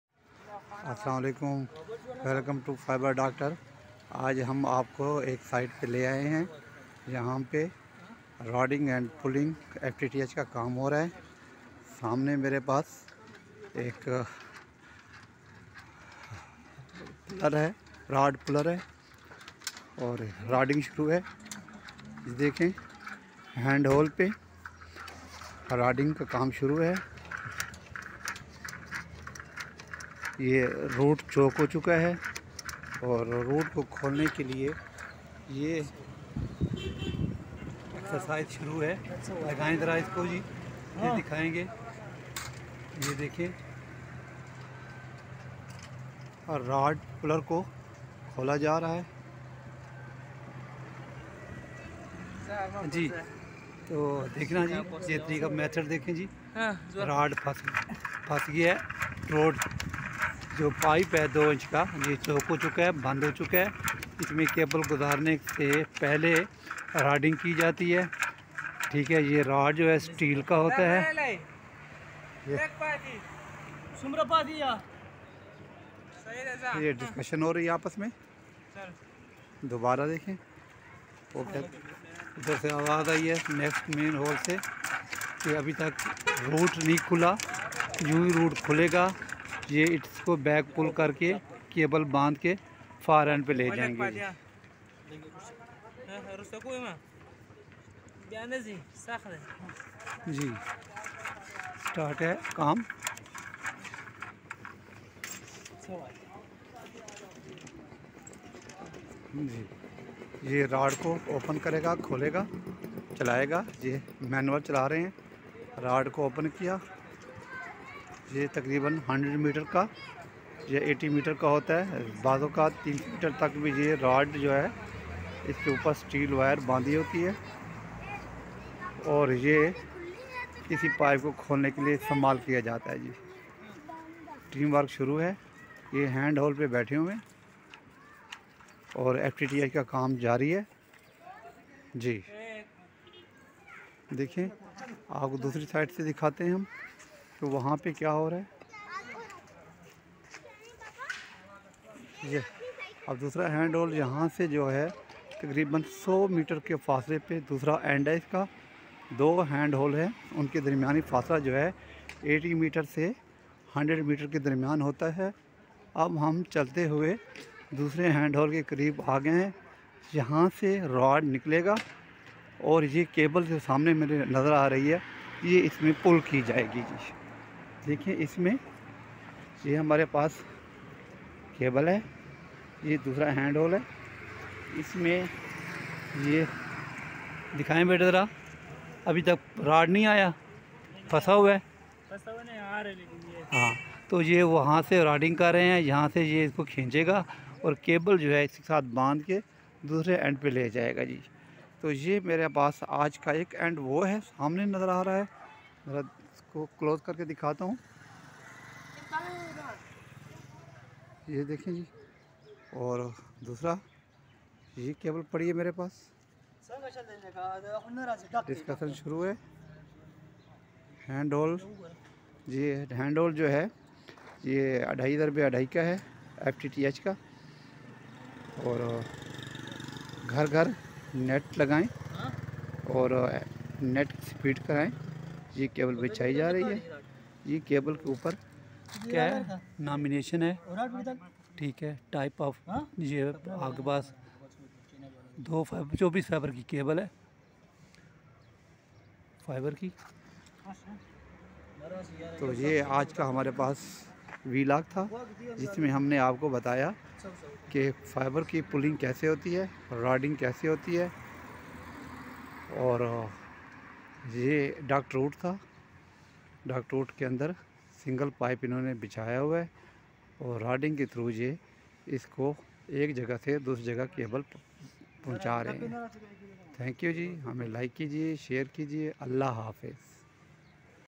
Fiber cable laying with duct Rod in ducting method
24Fiber  cable laying in Islamabad city with duct Rod methed

single duct route used

For FTTH, G-PON  installation
Hand holes 2x2 ft uses for local  connections connectivity